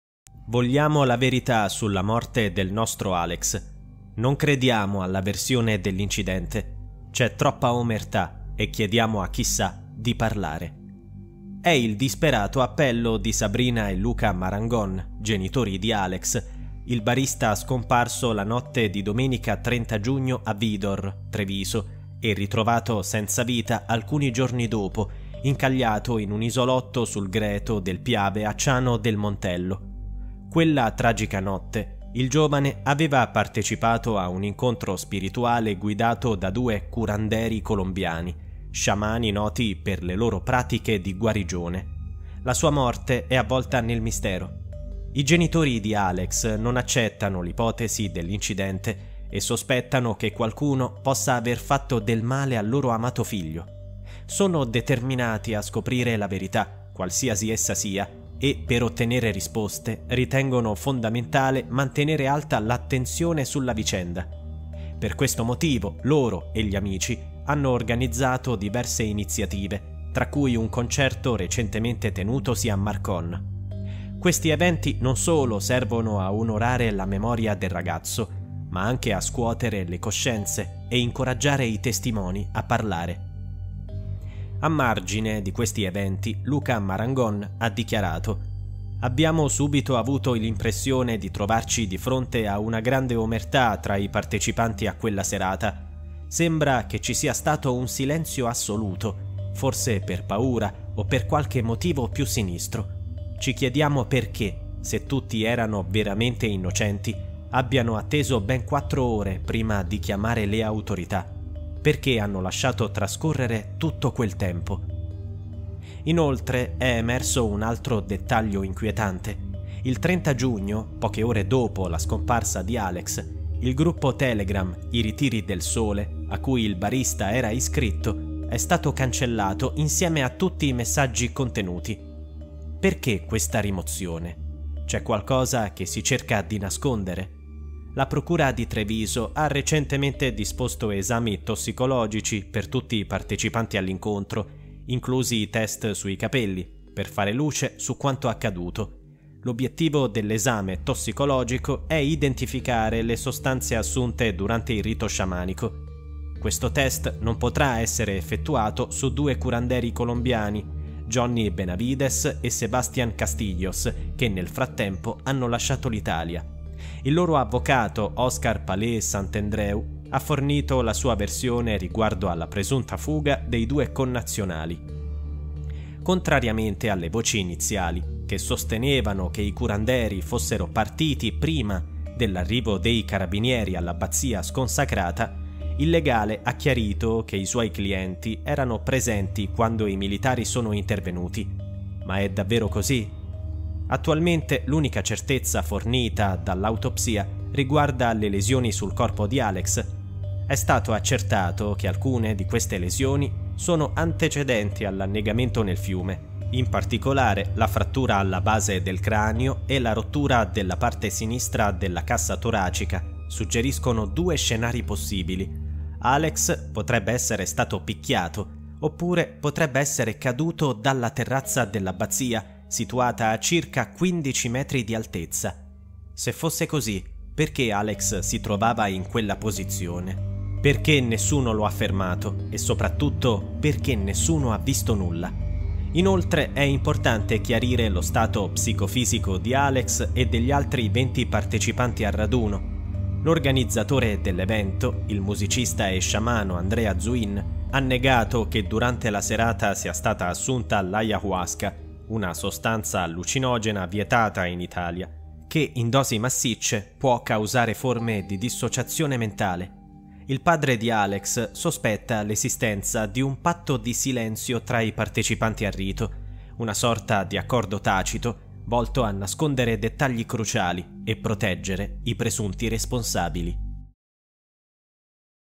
Test sui capelli - Alex Marangon
Proseguono le indagini sulla tragica fine di Alex Marangon.